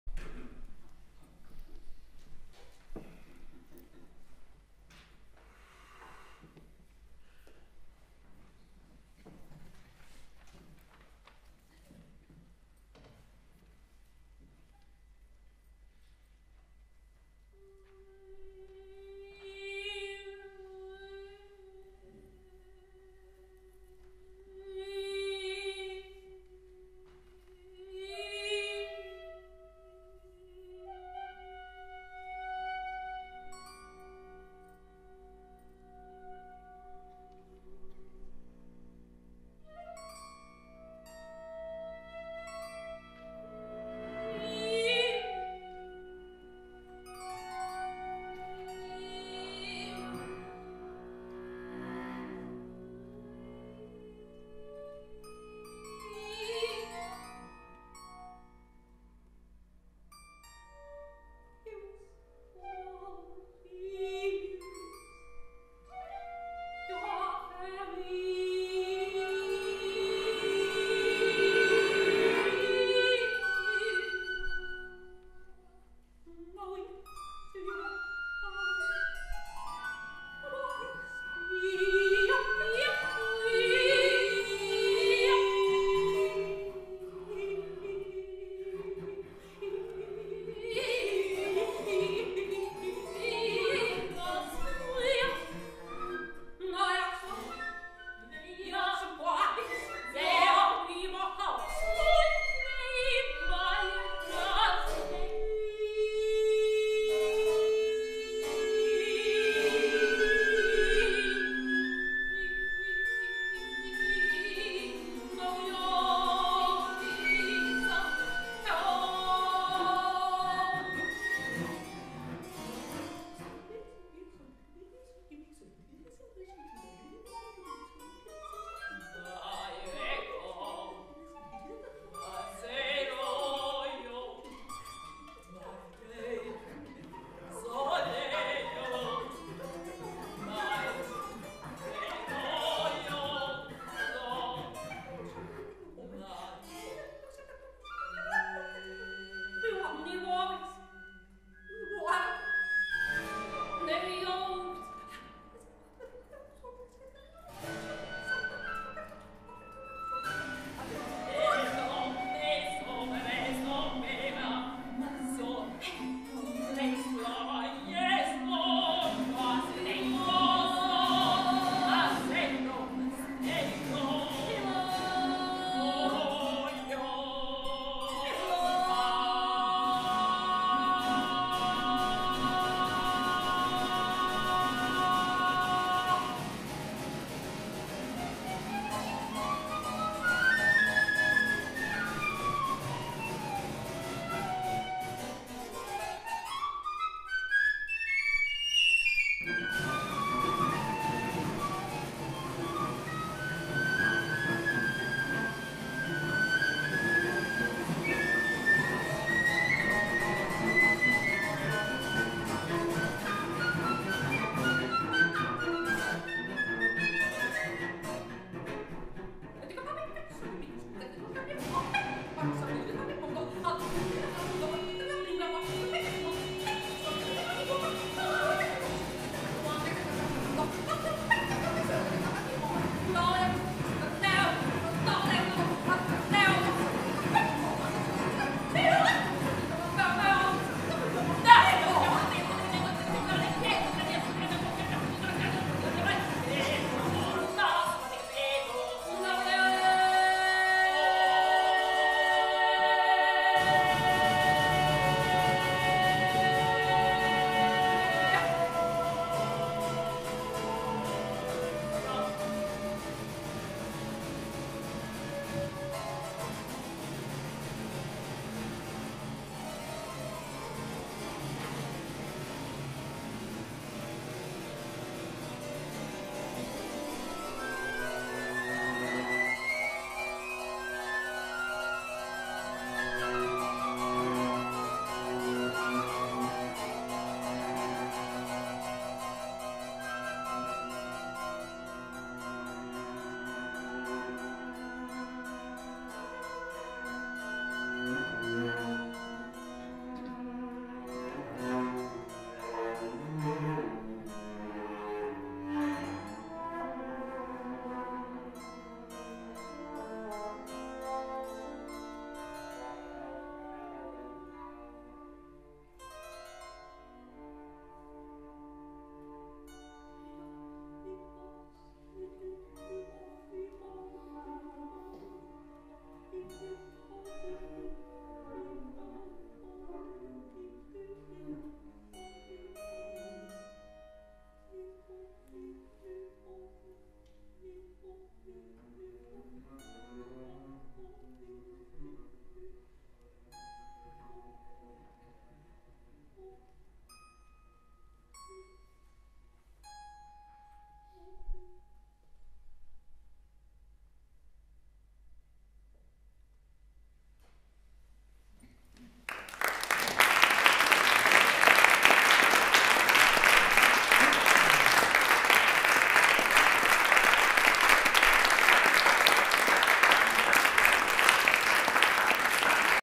A-L. Poll, S.Castrillon, M.Hrafnsdottir, K. Holsten & B.Seemann, 5.02.2016
Free improvisation by Anne-Liis Poll (voice), Marta Hrafnsdottir (voice), Klaus Holsten (flute), Sergio Castrillon (cello) and Beata Seemann (cembalo)  in Chamber hall of Estonian Academy of Music and Theatre, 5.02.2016. This performance was a part of the final concert of Erasmus + project METRIC (Modernising European Higher Music Education through Improvisation) Staff Training Event.